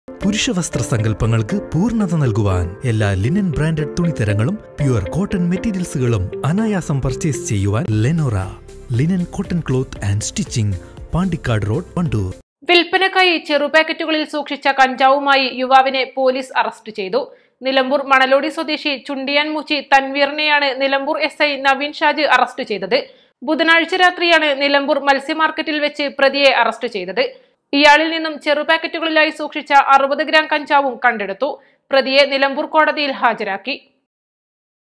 വിൽപ്പനക്കായി ചെറു പാക്കറ്റുകളിൽ സൂക്ഷിച്ച കഞ്ചാവുമായി യുവാവിനെ പോലീസ് അറസ്റ്റ് ചെയ്തു | Kanjavu
വിൽപ്പനക്കായി ചെറു പാക്കറ്റുകളിൽ സൂക്ഷിച്ച കഞ്ചാവുമായി യുവാവിനെ പോലീസ് അറസ്റ്റ് ചെയ്തു